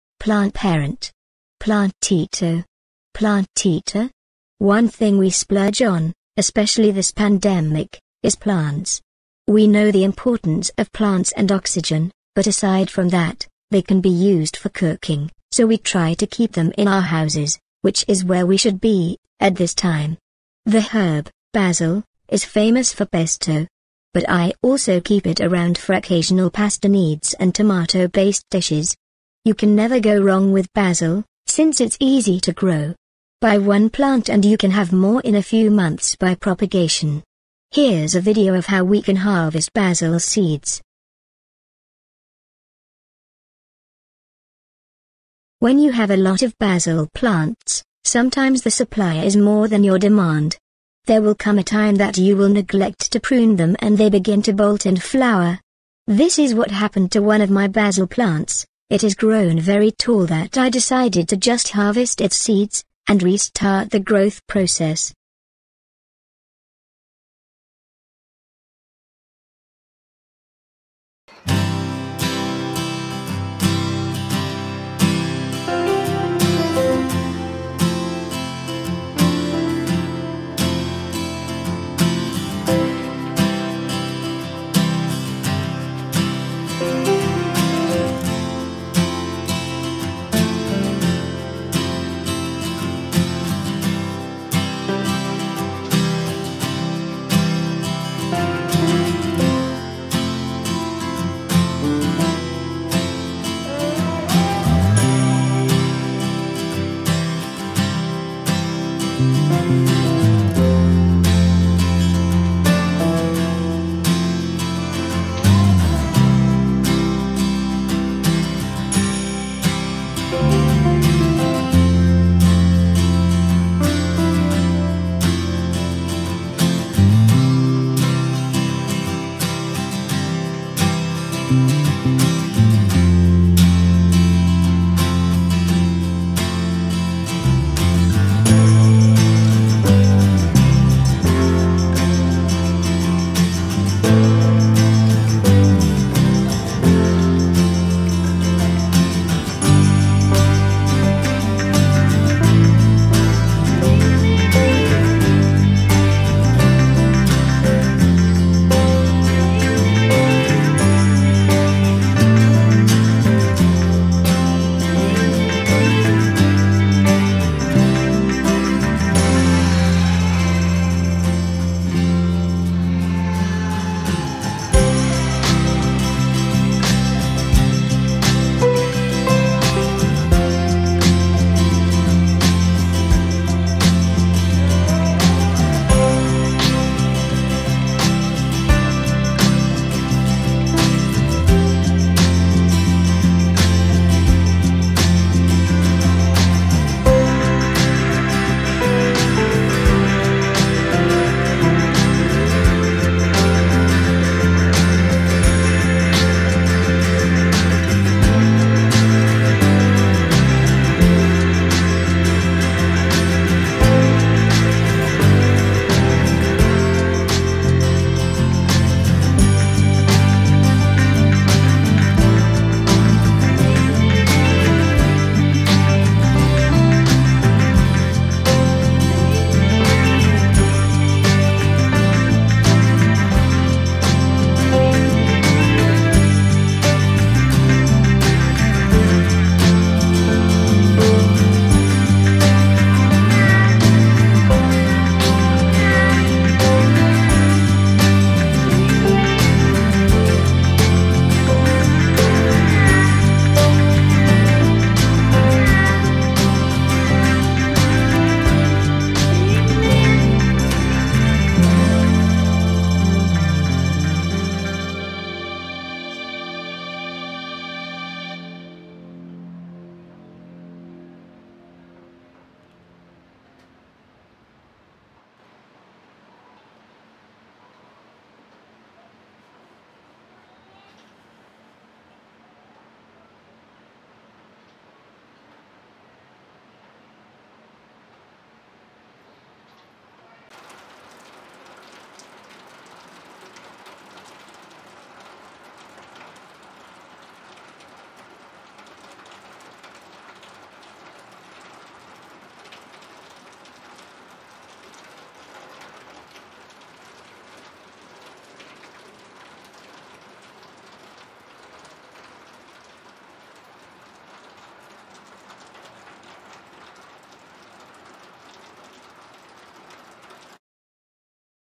Harvesting Basil Seeds
A video on harvesting basil seeds from bolted flowers.